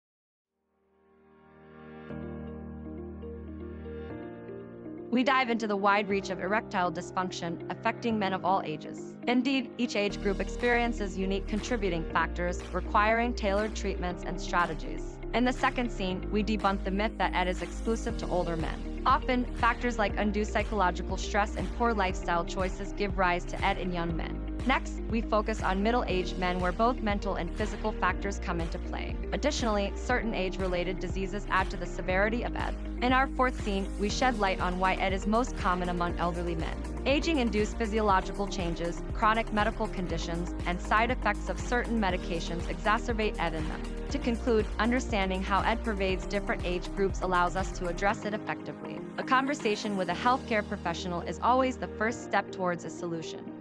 ERECTILE DYSFUNCTION Demystified - Prevalence and Management Across Ages | Mens Wellness
🔍 Curious about Erectile Dysfunction (ED)? 🚹 Wondering how it affects different age groups? Join us in this eye-opening journey as we delve into the realm of ED, its prevalence, and management strategies across various age ranges.

🔬 In our latest video, we dissect the complexities of ED and its impact on younger men, middle-aged men, and elderly gentlemen. Contrary to common misconceptions, ED isn't limited to just one age group. From psychological factors affecting the young bucks to the interplay of physical and emotional factors in the middle-aged, and the challenges posed by aging and chronic conditions in the elderly, ED's reach is far and wide.

💡 Our comprehensive analysis sheds light on the psychological stressors, hormonal imbalances, lifestyle choices, and underlying health conditions that contribute to ED. Discover how lifestyle modifications, counseling, medications, and even innovative devices come into play as management strategies.

👴🏼🧔🏻👦🏻 Whether you're a younger man seeking guidance, a middle-aged individual striving for balance, or an elderly gentleman navigating the challenges of aging, this video has something valuable for you. Remember, knowledge is power, and understanding the dynamics of ED can empower you to take charge of your health.

👨‍⚕️ Always consult a healthcare professional for personalized advice, but in the meantime, if you're eager to learn more, check out the full article